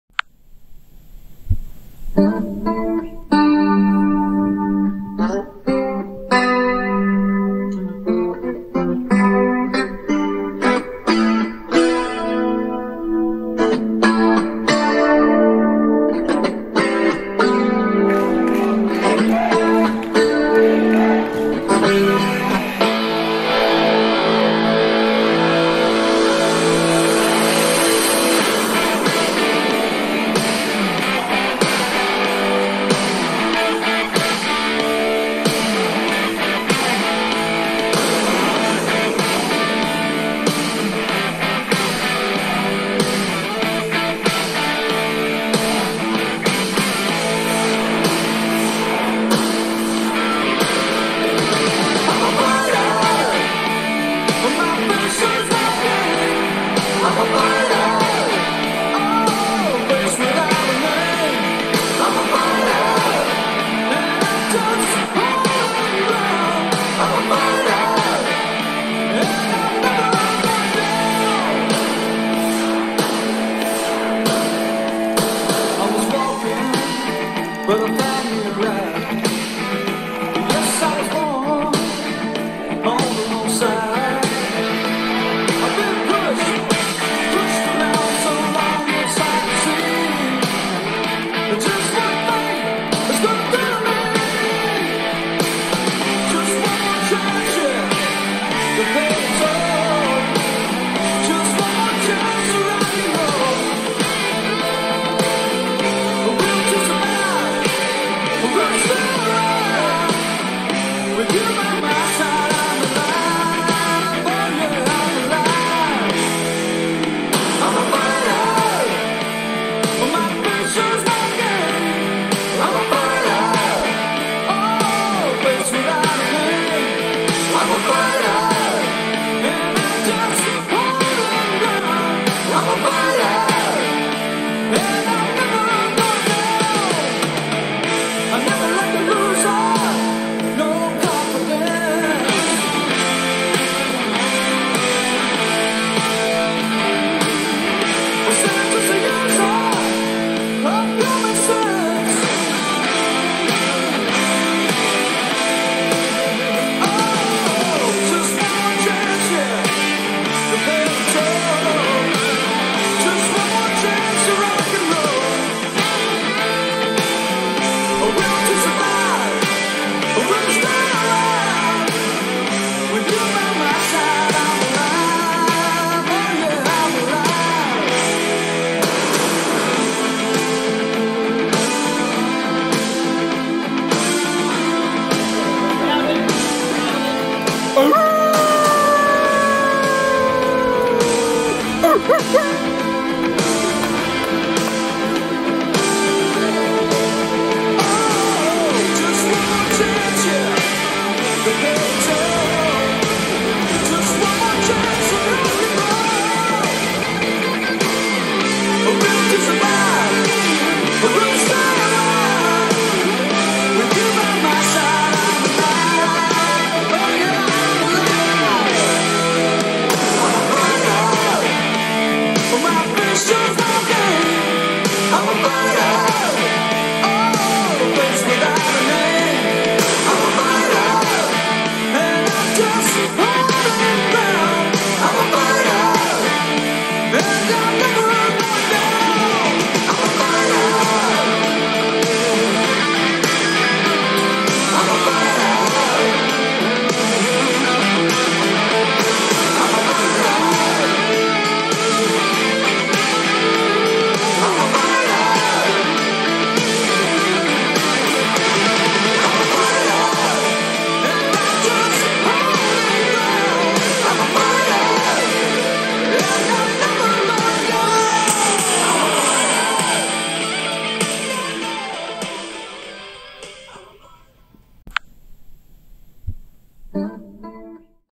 Liberty Creek Boys Basketball wins in a last half second thriller!!
Freshman Riley Clifton hits an incredible shot to seal the deal for the Wolves 44 to 42 over Montgomery Central in Clarksville.